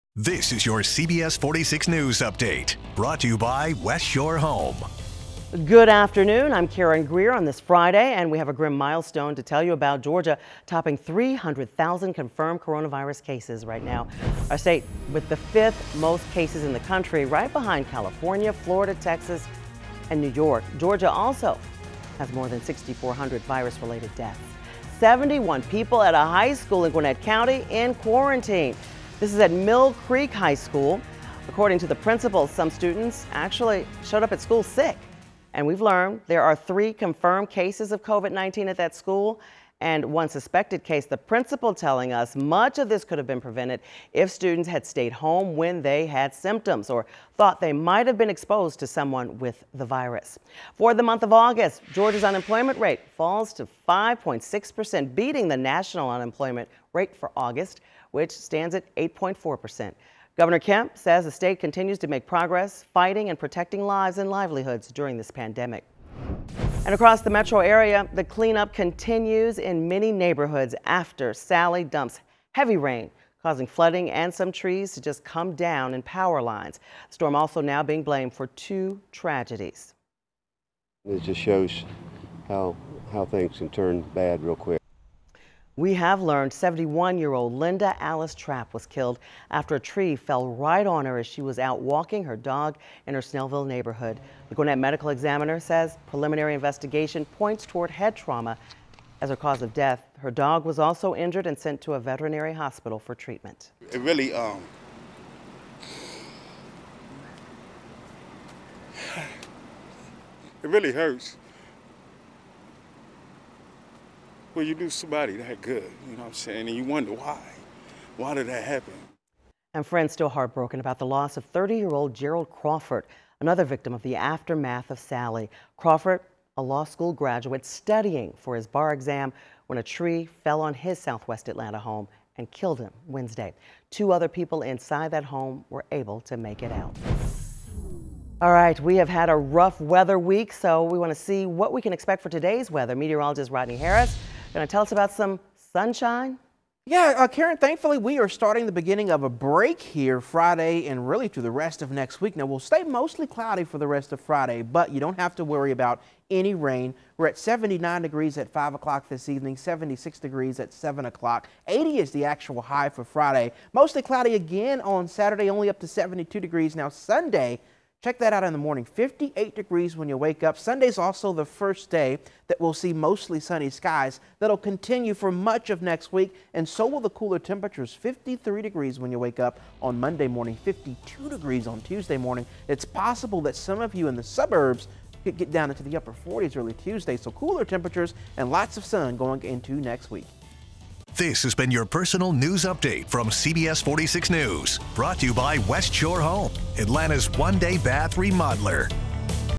CBS46 PM News Update 9/18/2020
Covid quarantine in high school

Unemployment drops in Georgia

Cleanup continues after Sally dropped heavy rain across Georgia

A taste of fall from Mother Nature this weekend